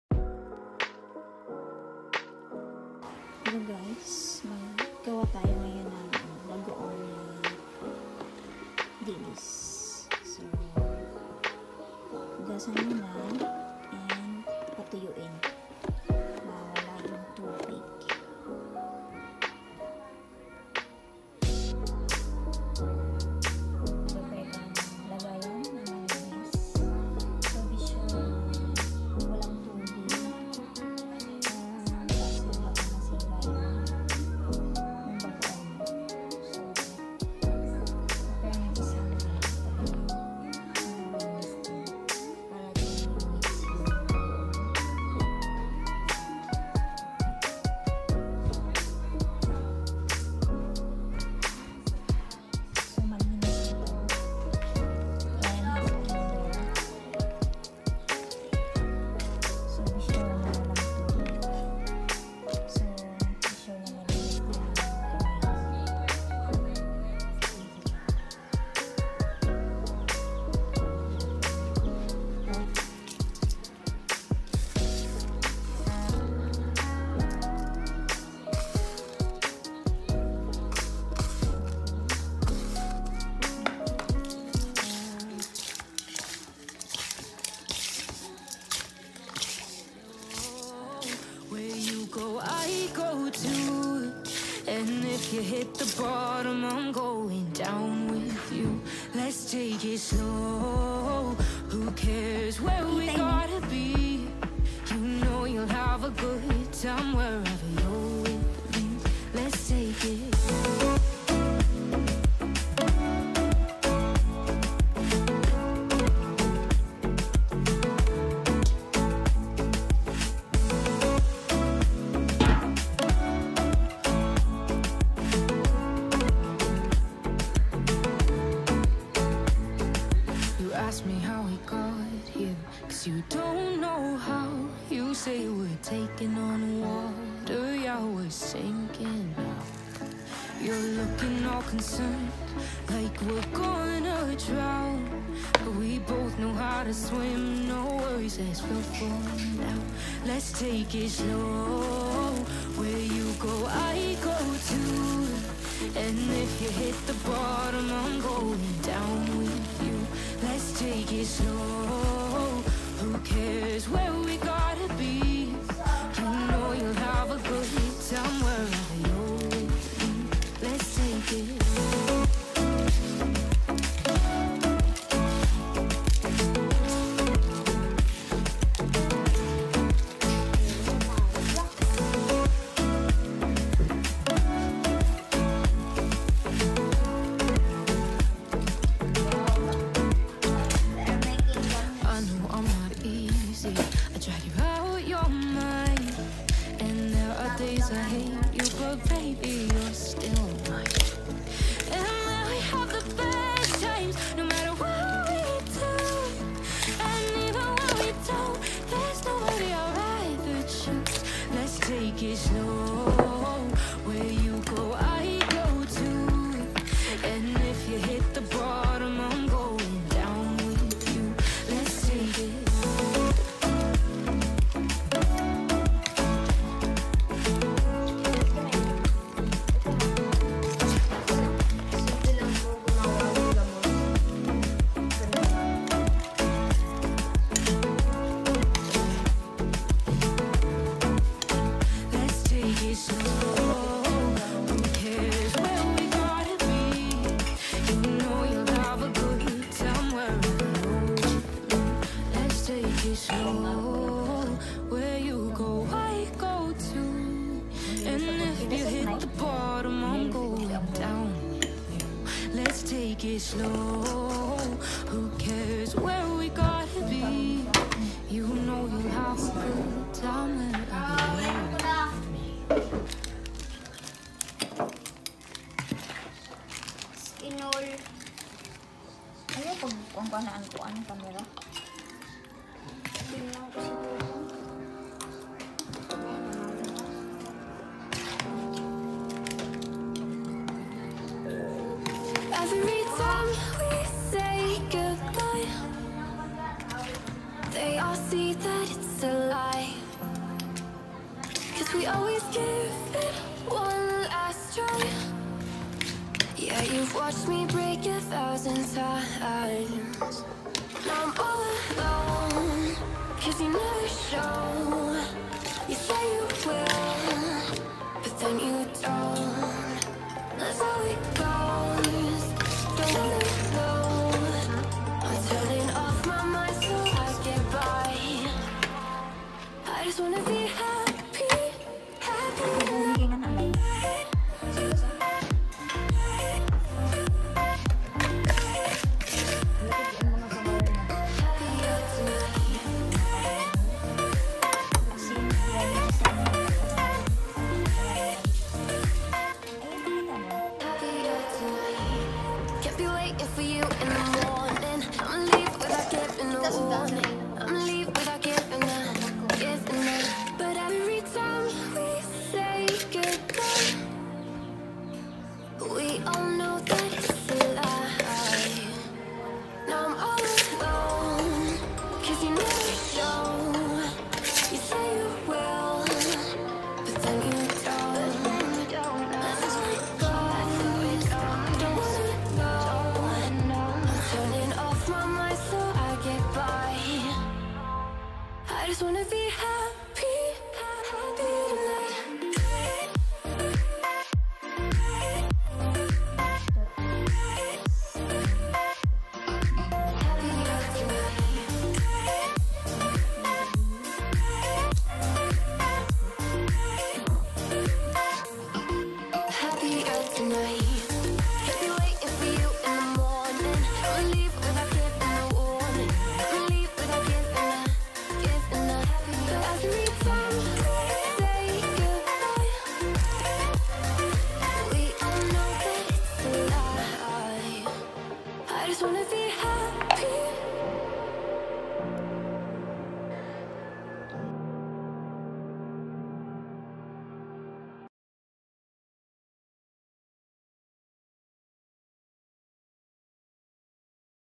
Homemade bagoong na dilis | Fermented anchovies | Salted small fish
Homemade Bagoong na Dilis;

Ingredients:

•1 1/2 kilo - Dilis | small fish | anchovies
•1/4 rock salt
•3 Tbsp rock salt - for cleaning the fish with water.

1. Clean the fish in running water
2. Use the strainer to remove the excess water.
3. Put 3 Tbsp rock salt then leave it for 30 minutes.
4. Then strain it again to remove the excess water.
5. Then  put the 1/4 kilo rock salt ( add more salt for your preferred taste)
6. Put it in a bottle jar.

The Bagoong na dilis can be served as early as 2 weeks and onwards,it depends on you. Store it in a cool and dry place and away from heat or direct sunlight.